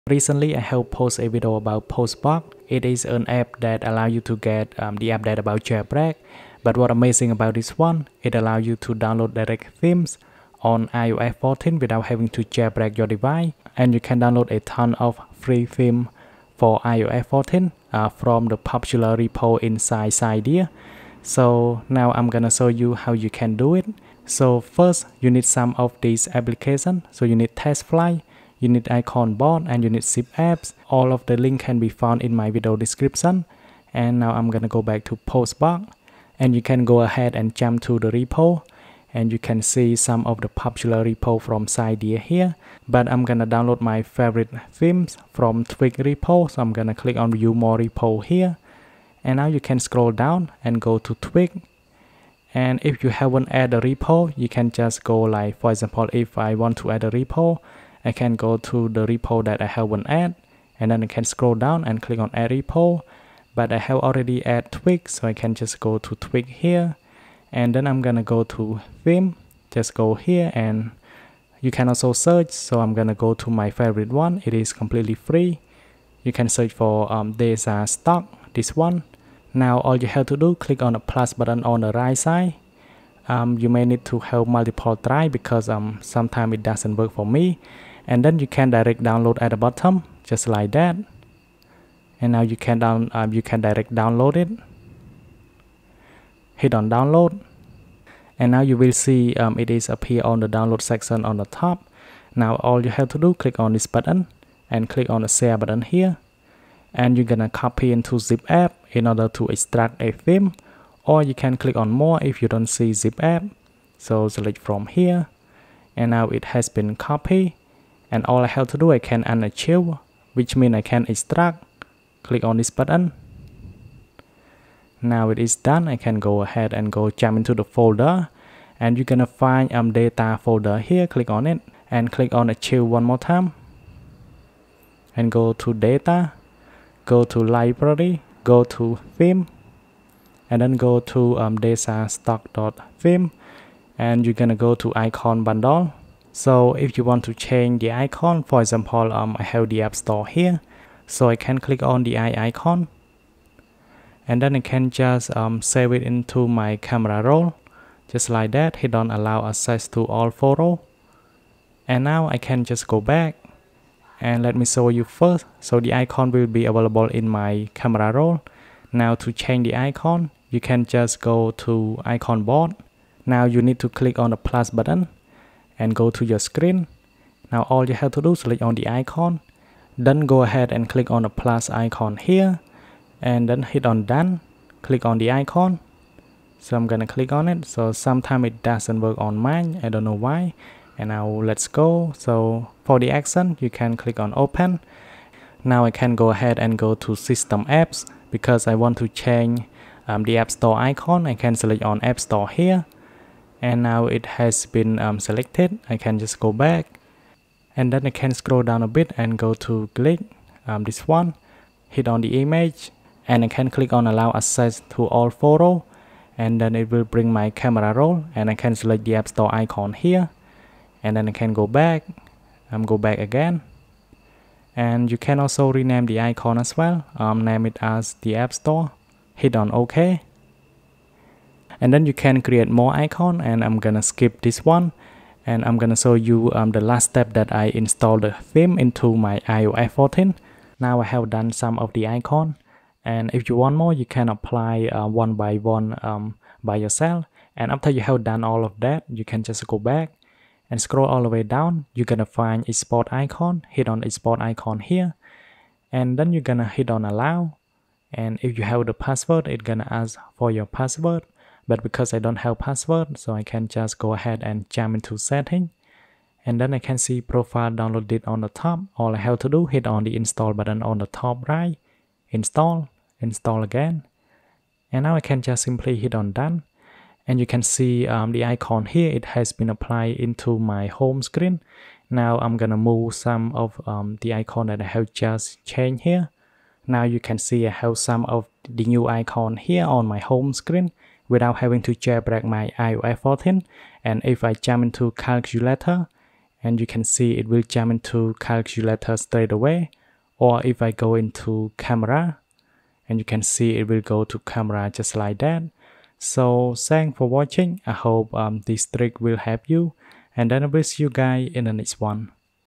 Install FREE JAILBREAK THEMES No Jailbreak ON iOS 14
With POSTBOX it is amazing. You can direct download theme from popular repo. And apply on iOS 14 with no Jailbreak

Thank to @TheiDeviceBlog He found this trick :)